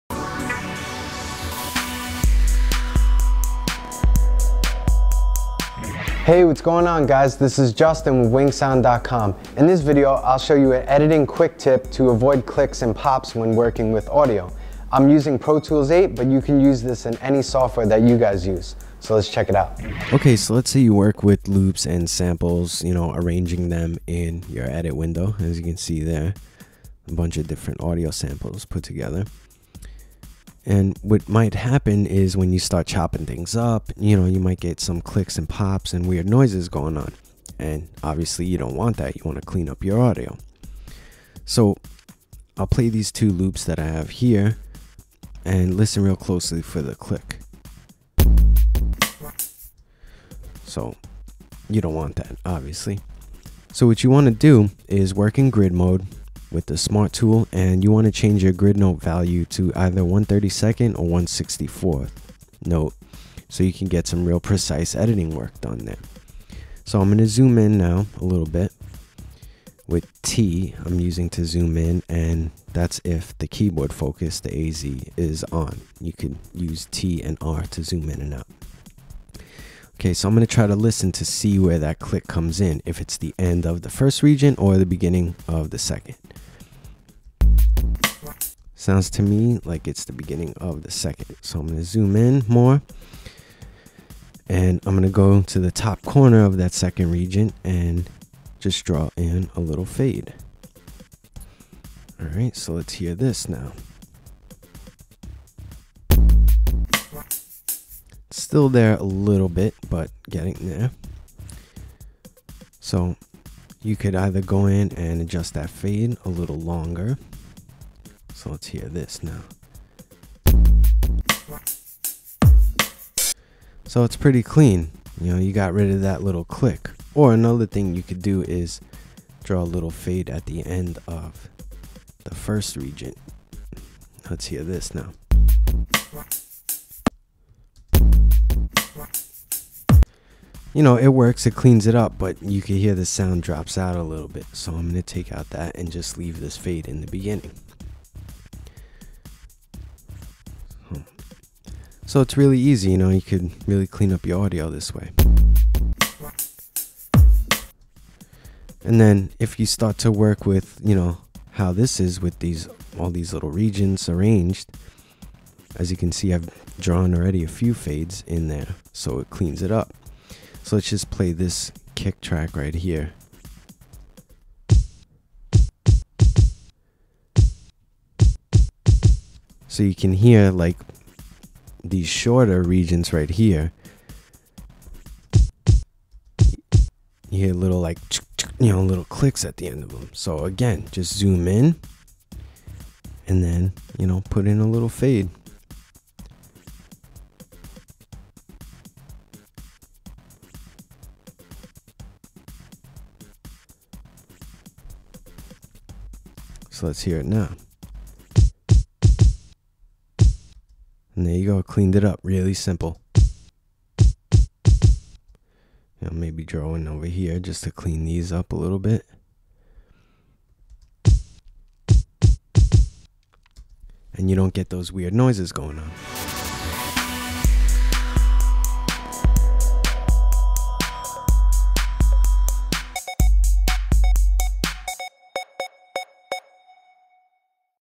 Pro Tools: How To Avoid Clicks and Pops when Editing Audio | WinkSound
When editing audio files one can run into clicks and pops and the beginning or end of the region. This tutorial explains how to avoid these and any other artifacts when editing audio within Pro Tools 8.